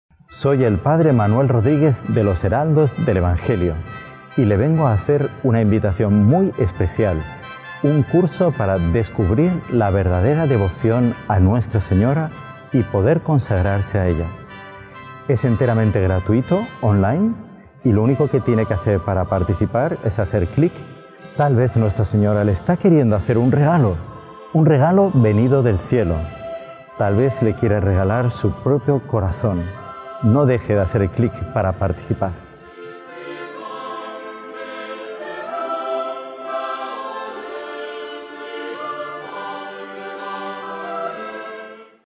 Curso de Consagración a la Virgen
Participa en el Curso de Consagración a la Virgen de los Heraldos del Evangelio.
Iniciamos el 13 de febrero 2023

✥ Este curso es 100% gratis y 100% en línea.

✥ Clases con contenidos exclusivos.

✥ Recibirás todos los días un link para entrar en la clase y los textos correspondientes.

✥ Vídeos y audios de alta calidad.

✥ Podrás también hacer tus preguntas por correo electrónico.

✥ Al terminar el curso, harás tu consagración y recibirás tu certificado.